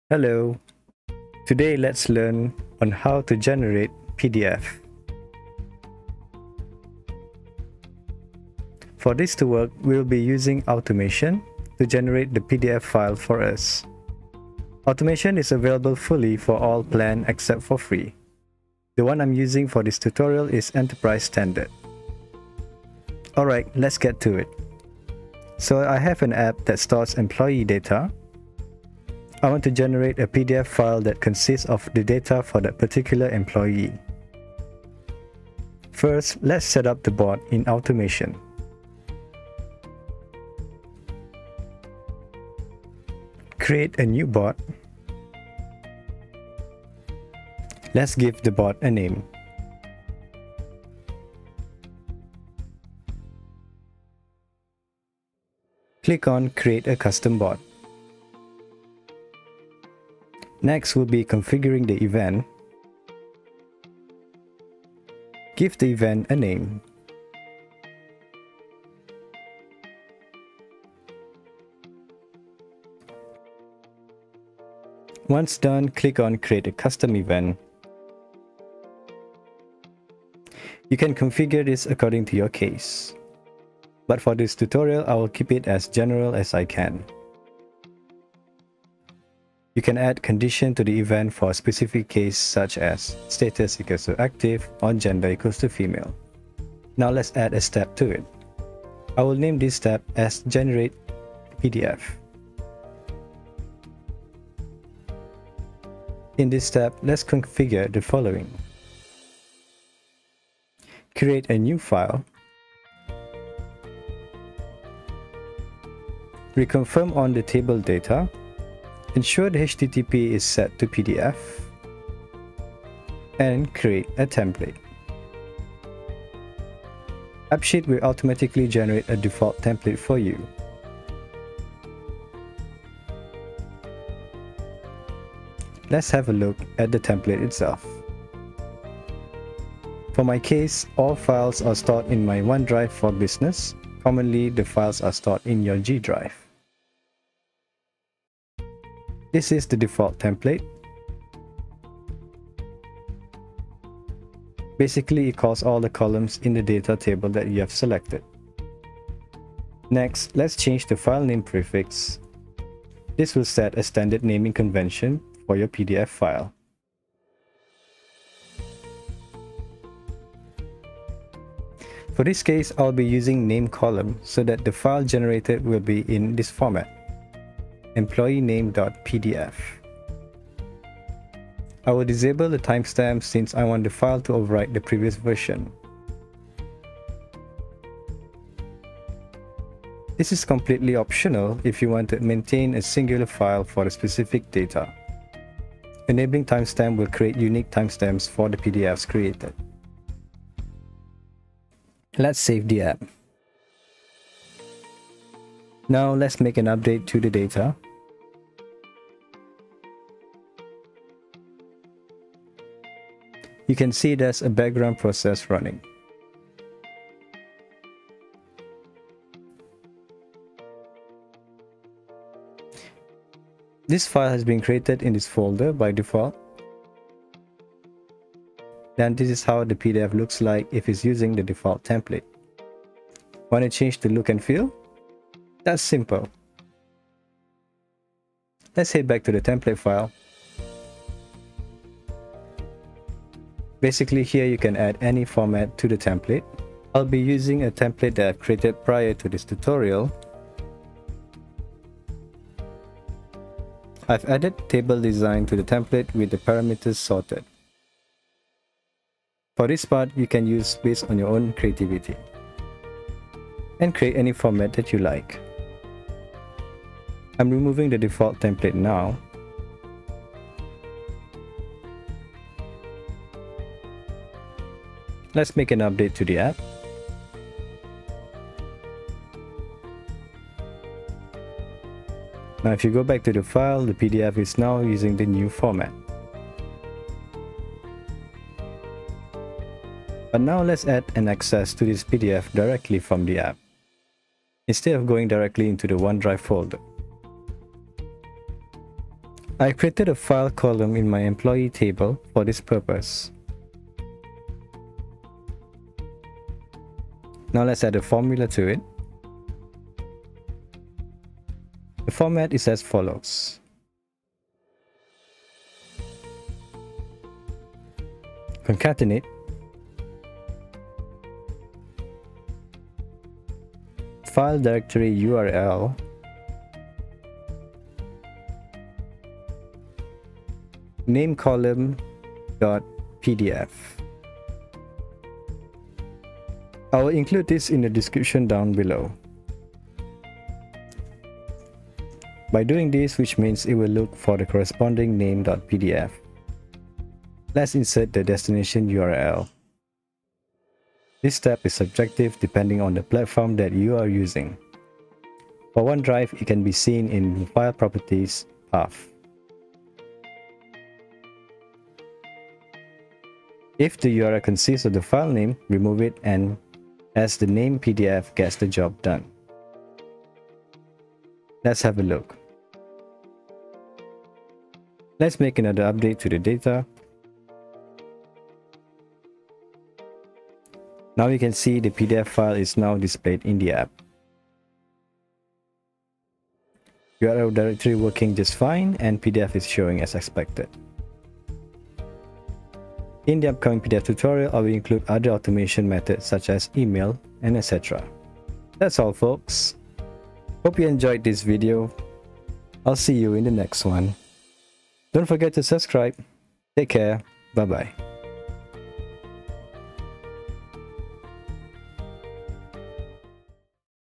Appsheet Episode 15: How to generate PDF using Automation.
In this video, I'm sharing how to generate PDFs using Automation in Appsheet. By using Automation, you'll be able to streamline your PDF creation process and create PDFs quickly and easily.

If you're part of a team that needs to create PDFs frequently, then this video is for you. By learning how to use Automation in Appsheet, you'll be able to create PDFs quickly and easily, saving you time and money in the long run.

Expression used :
CONCATENATE("FILE URL",[Name],".pdf")

My channel is about Technology and General videos. We cover lots of cool stuff such as Appsheet tutorial, highlights and day to day stuff.